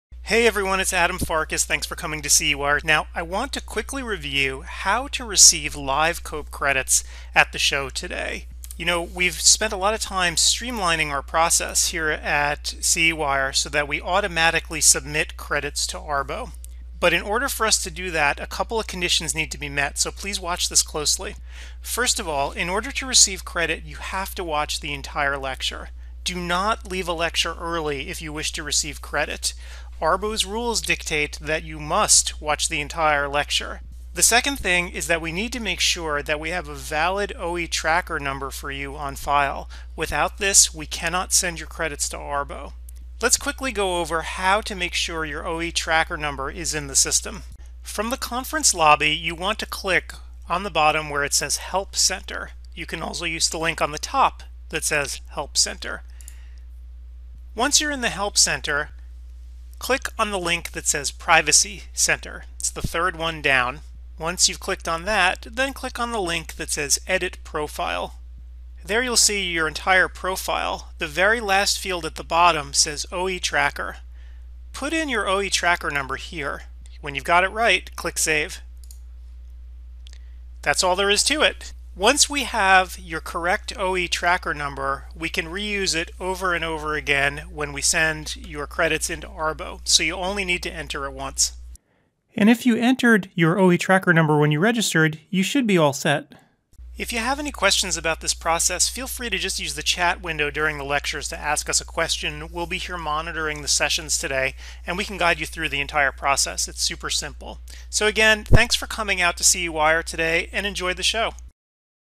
How to get COPE Credit at CEwire2021: Walkthrough
Make sure your OE tracker # is in place, so we can automatically send your credits in to ARBO!

ODwire.org: The Largest Online Community Exclusively for ECPs.
Always Free * Always Fun!